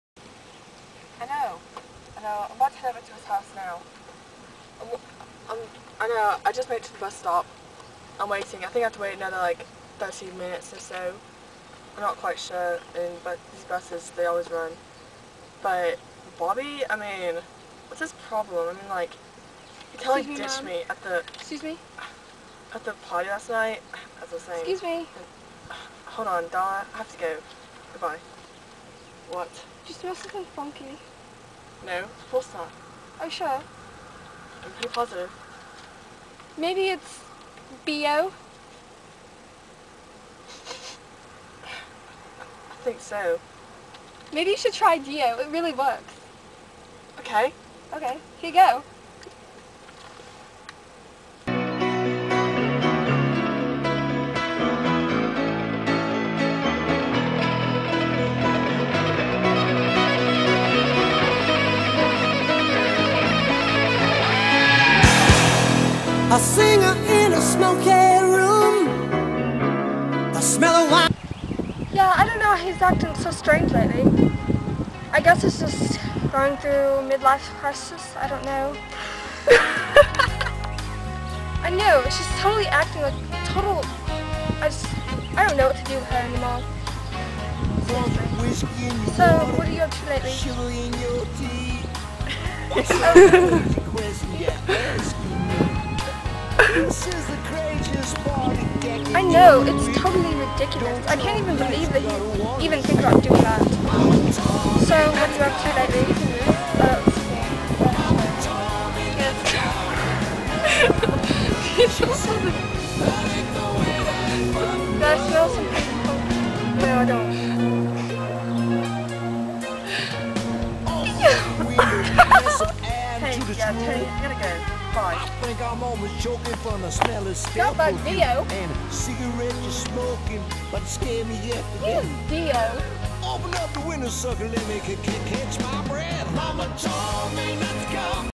Deo Commercial
Some British Girl decides to advertise Deo brand by going up to random people.

our first video, CMNT PLEASE!